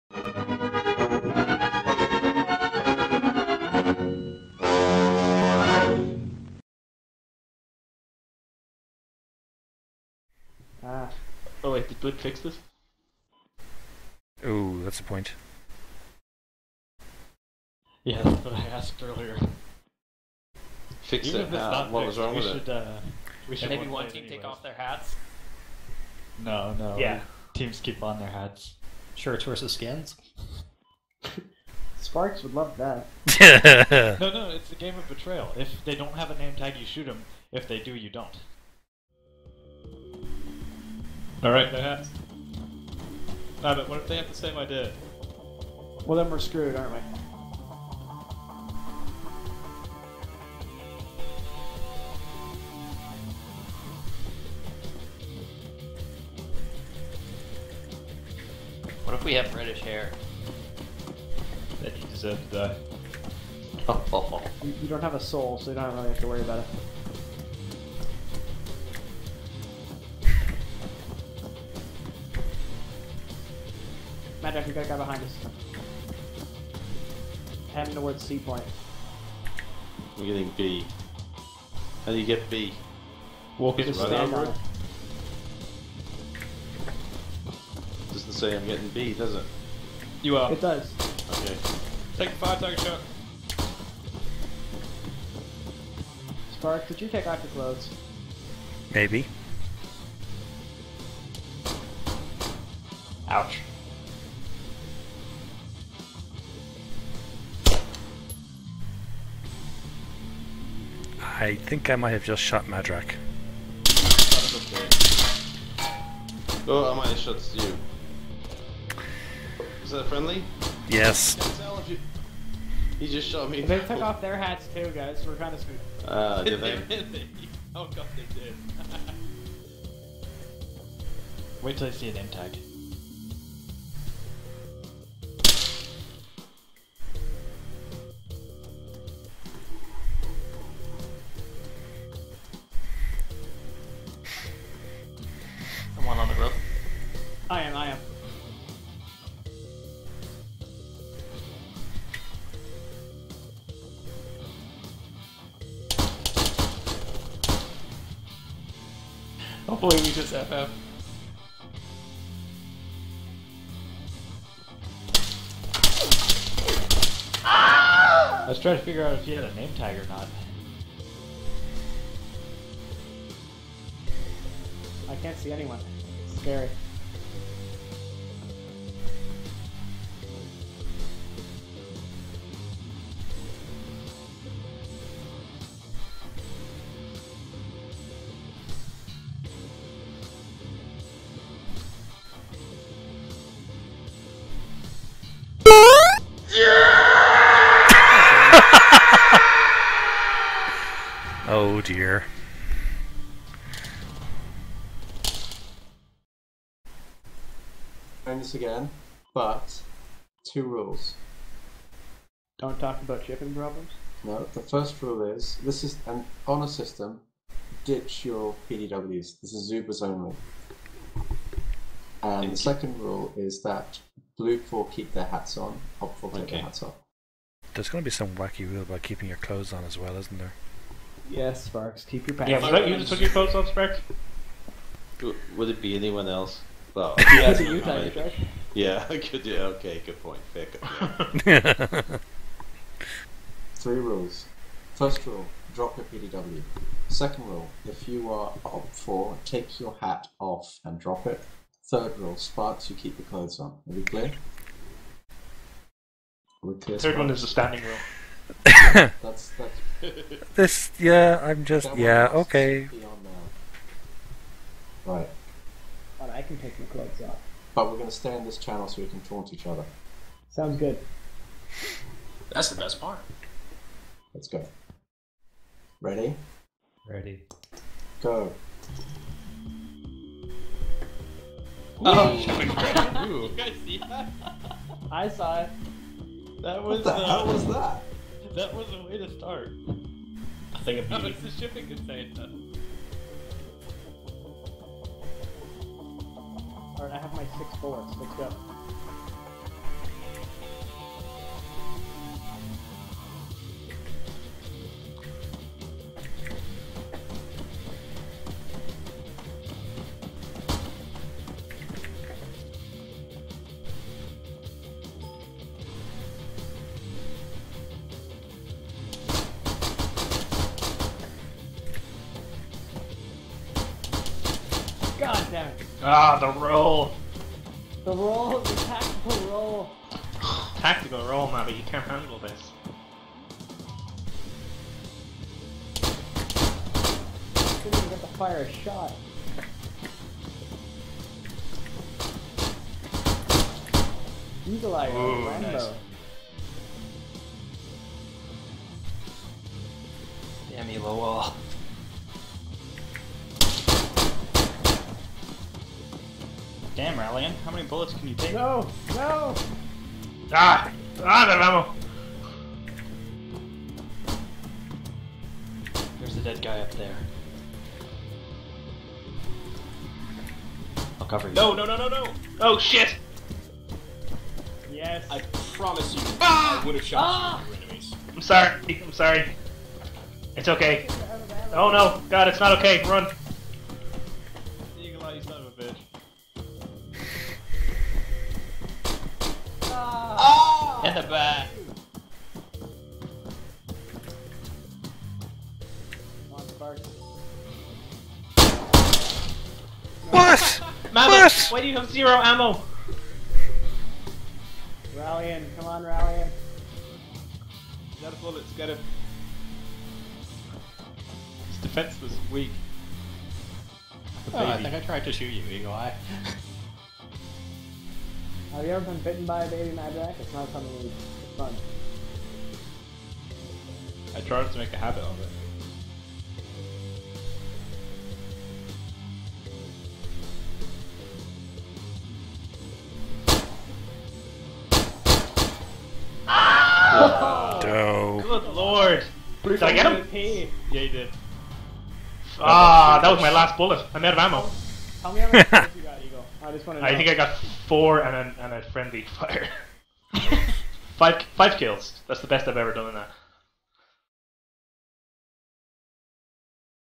Folk ARPS 13 September 2015 - Shipping Problems
Afterparty fun from the Folk ARPS session on Sunday 13 September 2015 in the still-not-fixed-yet Shipping Problems mission.

I'm starting to think we might have to fix this one...